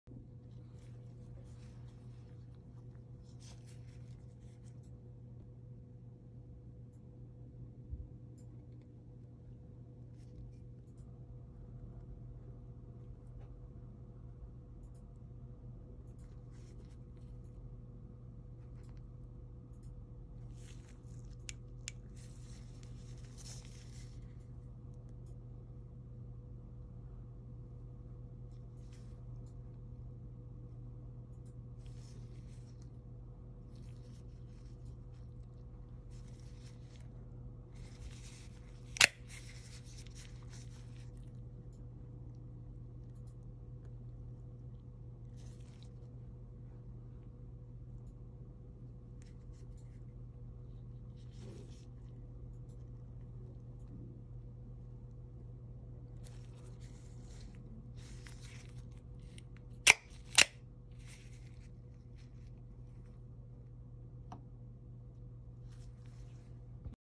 G&G Hawk Custom Deadlock Model “C” DLC Blade and Chassis with Yellow Matter Fat Carbon
G&G Hawk Custom Deadlock Model “C” DLC double edge blade, DLC titanium chassis with yellow matter fat carbon inlays and DLC hardware.